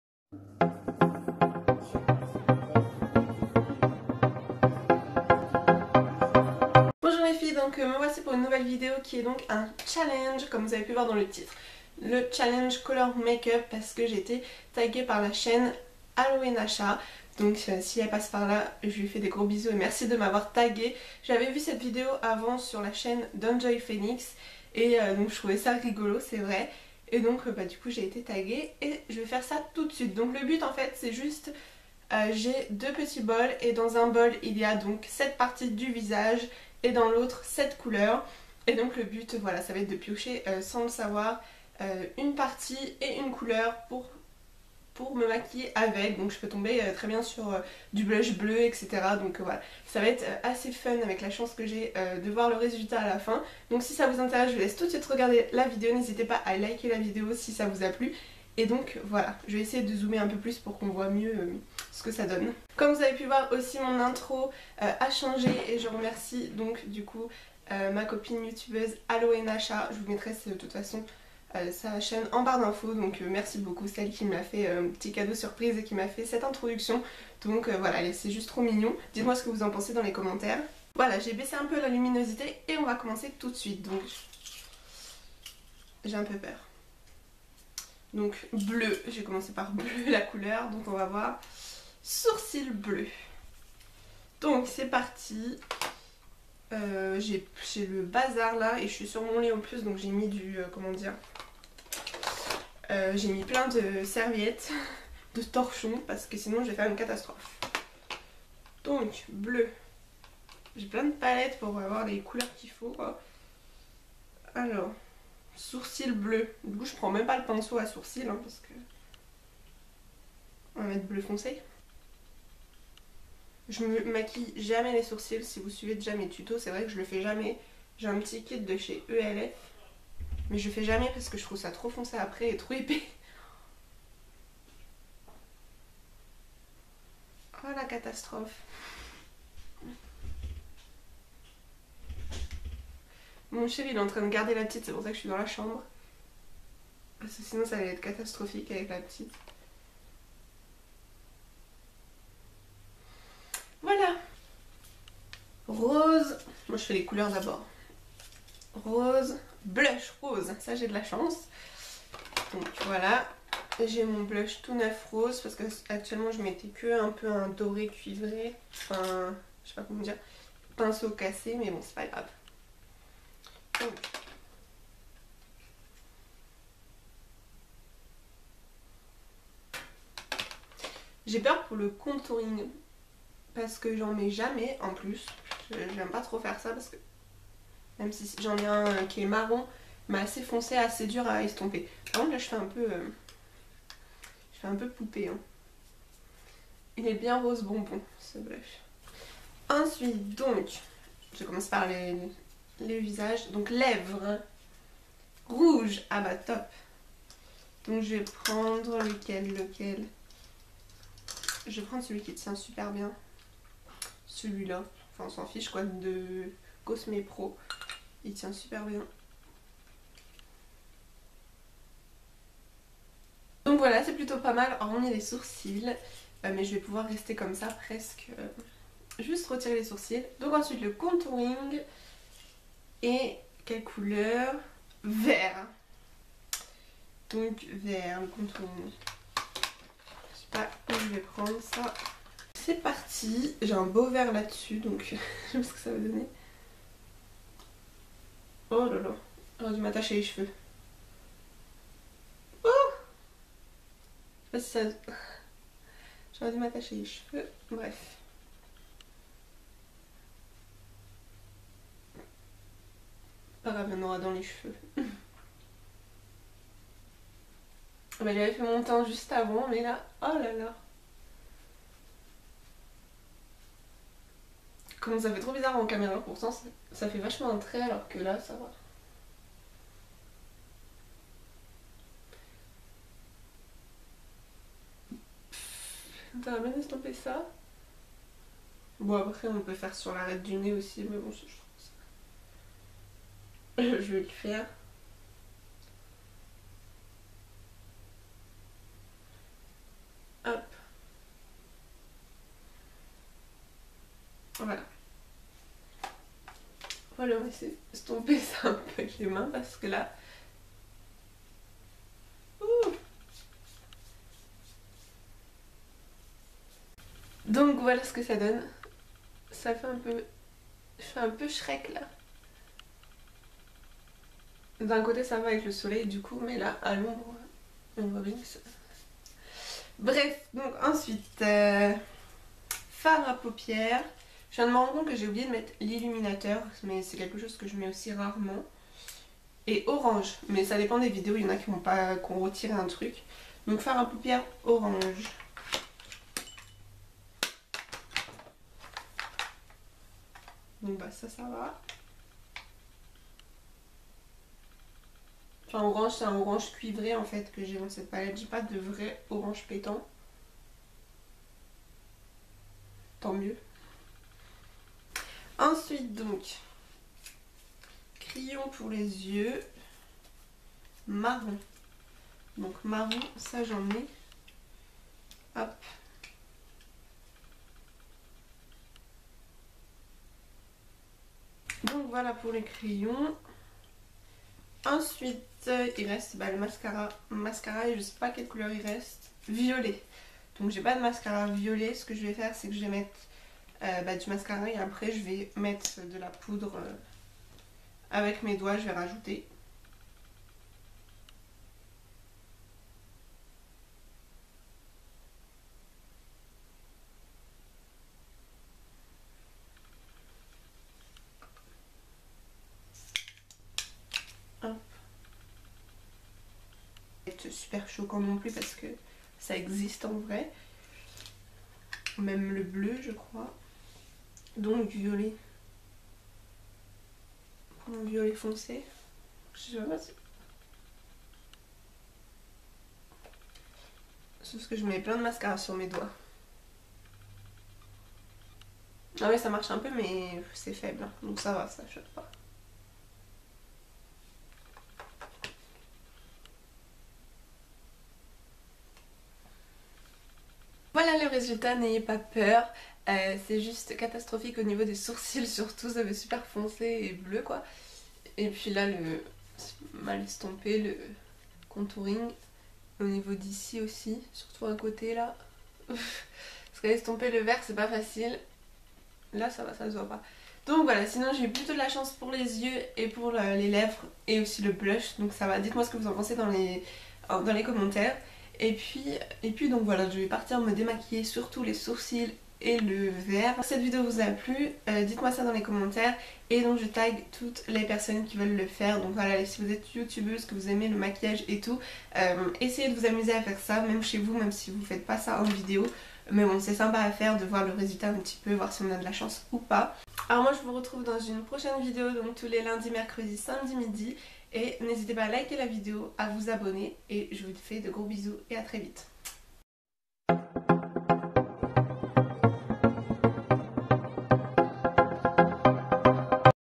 [CHALLENGE] COLOR MAKE UP
Coucou les filles!! J'espère que cette vidéo challenge vous plaira c'était fun à filmer, on en parle des sourcils? OMG^^
Désolée pour le flou.. Si je refilmer une 2e fois ca aurait été de la triche.

Je taggue toutes les youtubeuses, et meme toi qui lit cette barre d'info !!

Bisous!